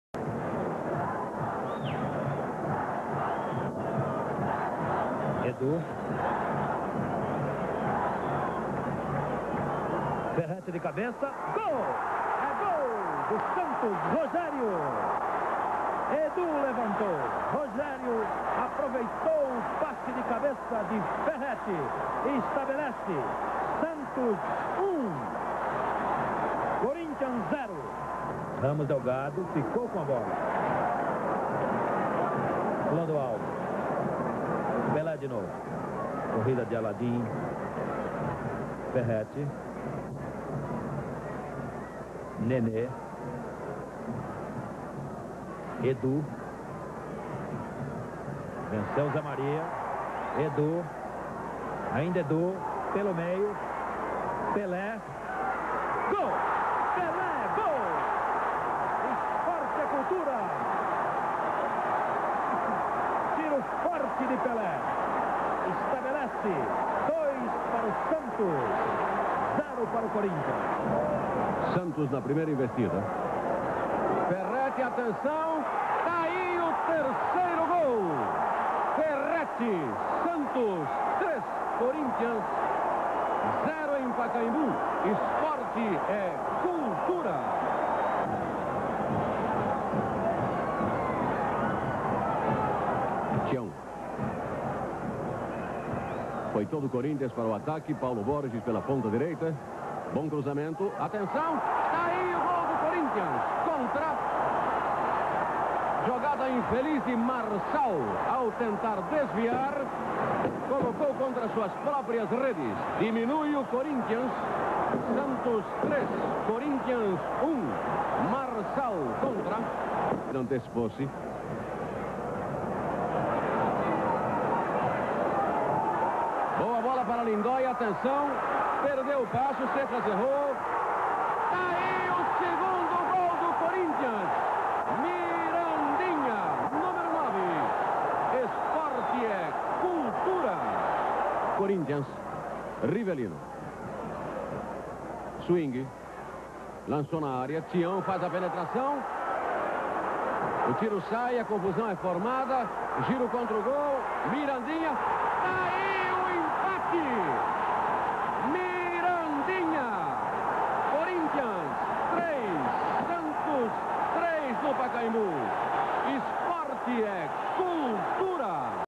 Santos 3 x 3 Corinthians Paulistão 71
Santos 3 x 3 Corinthians
20/06/71
Estádio Pacaembu
Público 29.502
Gols - Rogério(S), Pelé(S), Ferreti(S), Contra(Cor), Mirandinha(Cor), Mirandinha(Cor)

TV Cultura
Narração - Orlando Duarte e Luis Noriega